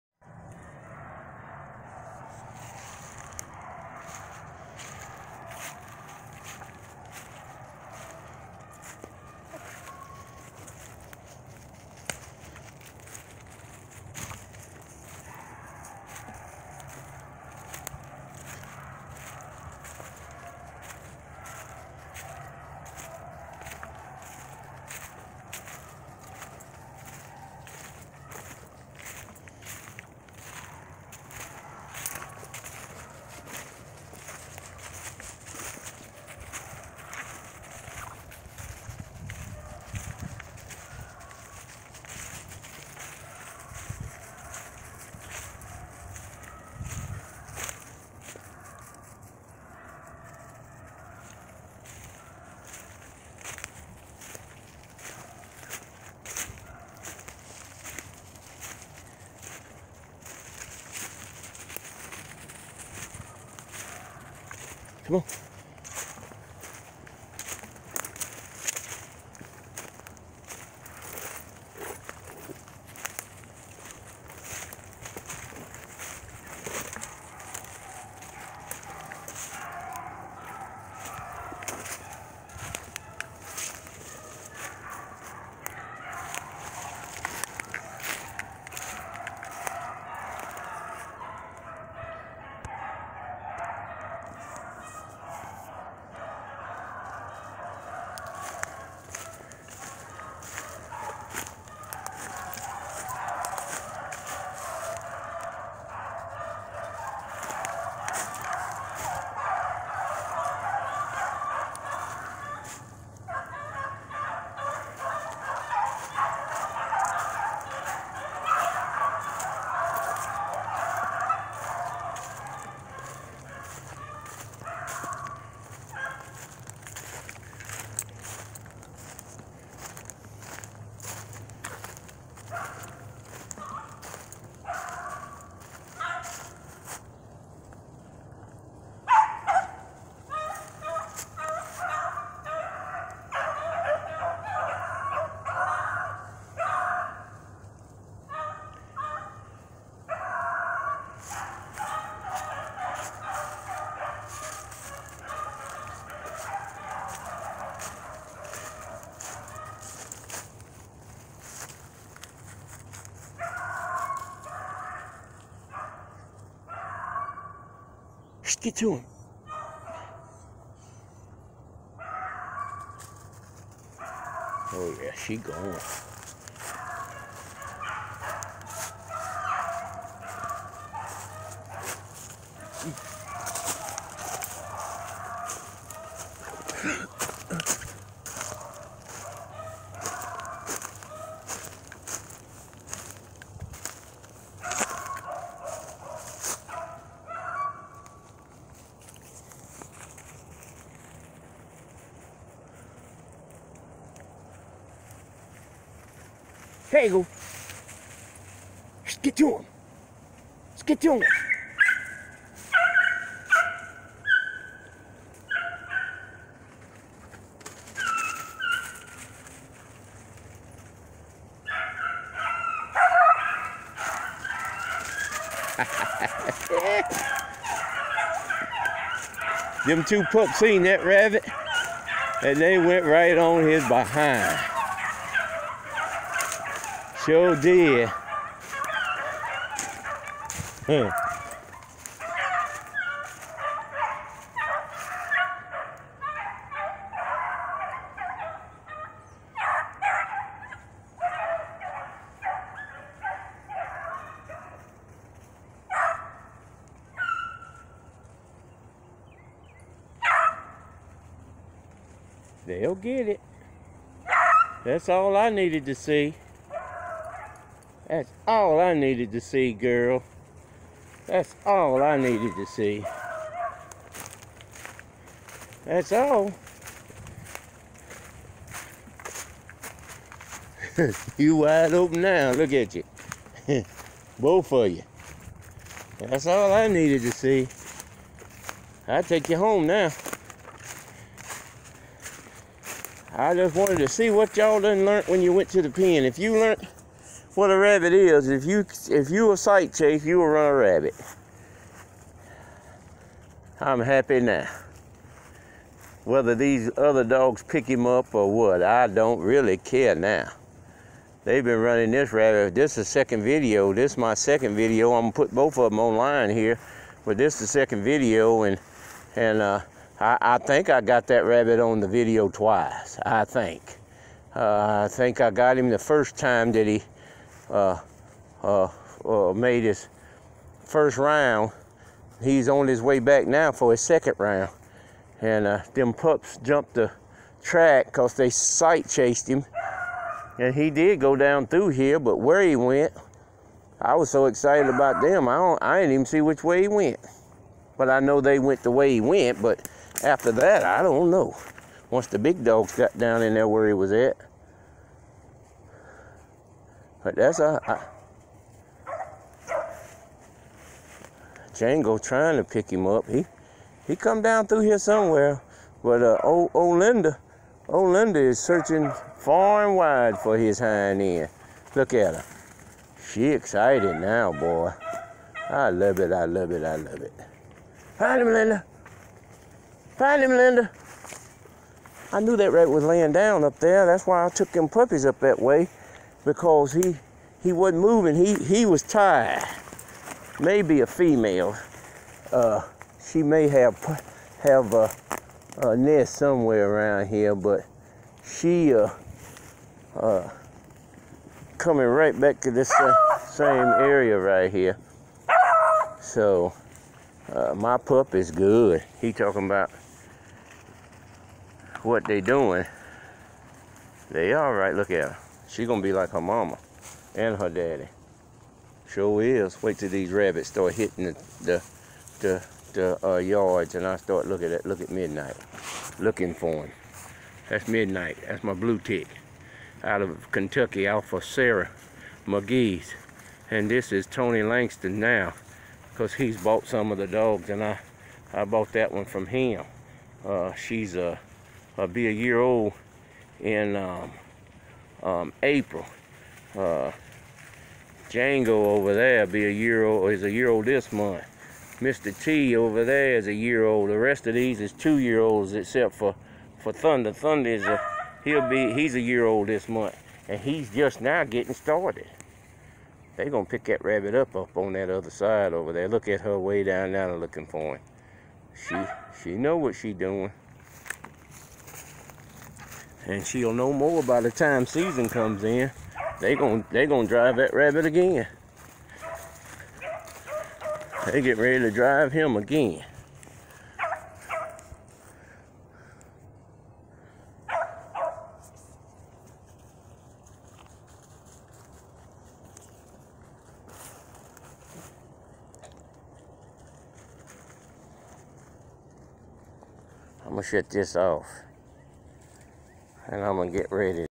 Homeboyz Hunting Club
At the end of race pups c rabbit and sight chases